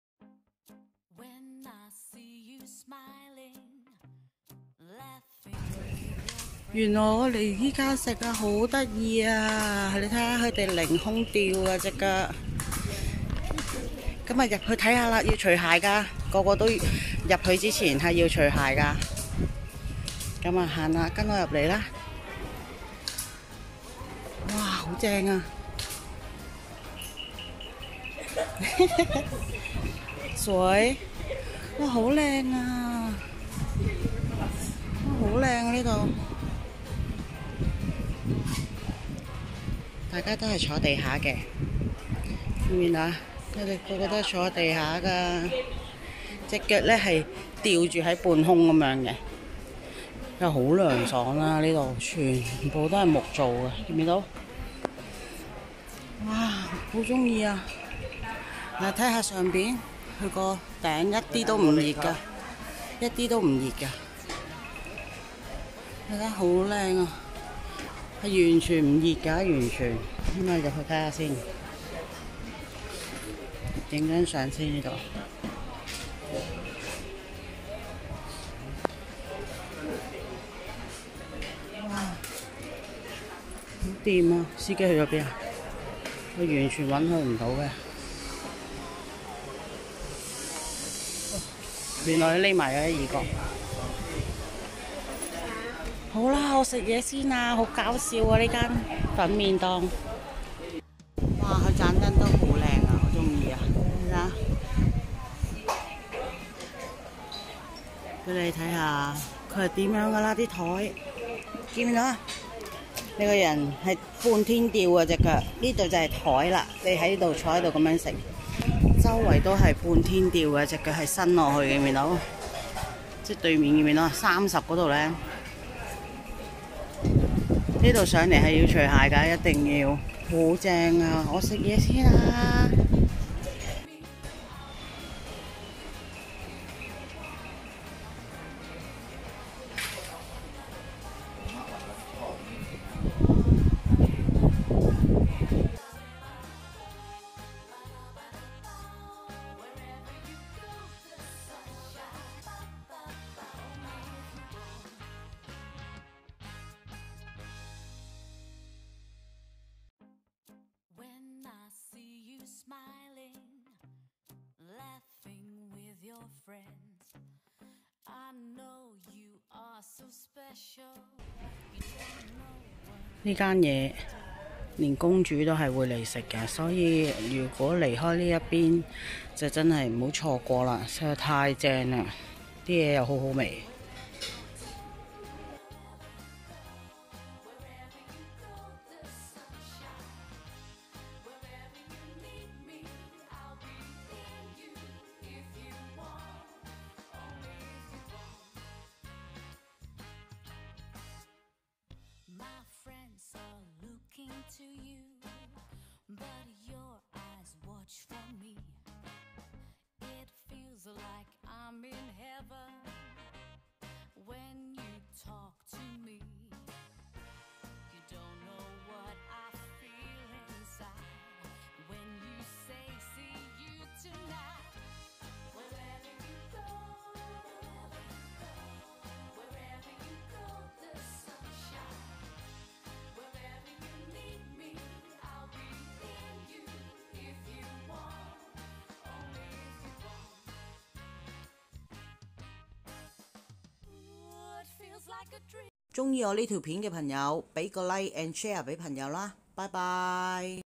Phitsanuiok😍彭世洛府呢間麵店好正
佢整間麵店都係木做
就算大熱天時都唔會熱
就係成功佛廟後面
有機會大家黎呢個城市一定要黎啊